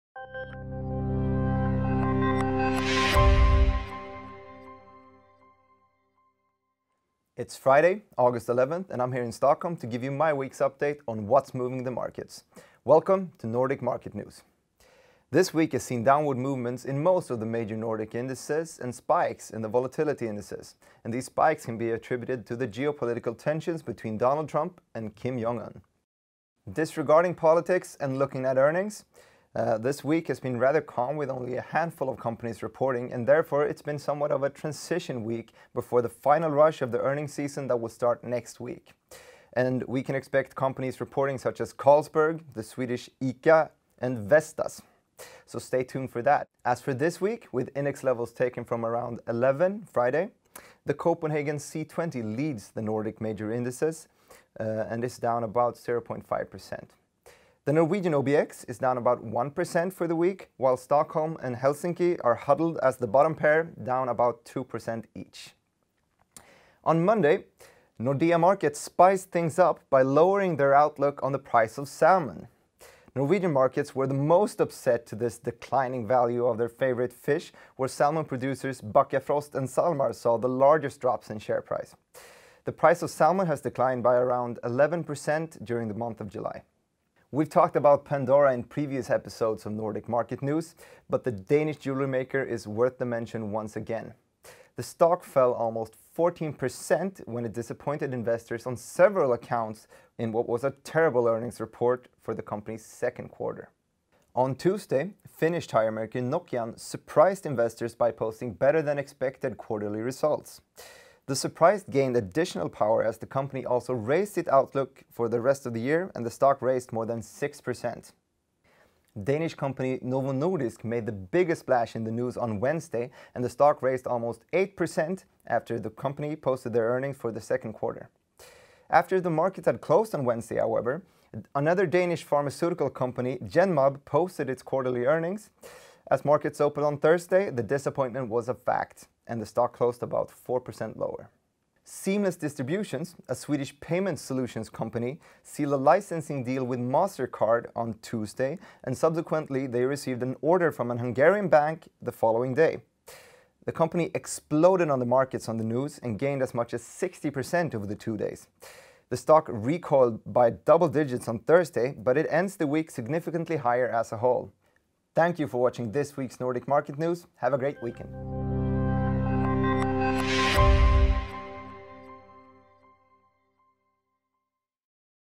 NMN 2017-08-11 Nordic markets fall in week plagued by North Korean crisis
Nordic Market News gives you the important market-moving news from the Nordic markets. Produced weekly by Nyhetsbyrån Direkt for Infront.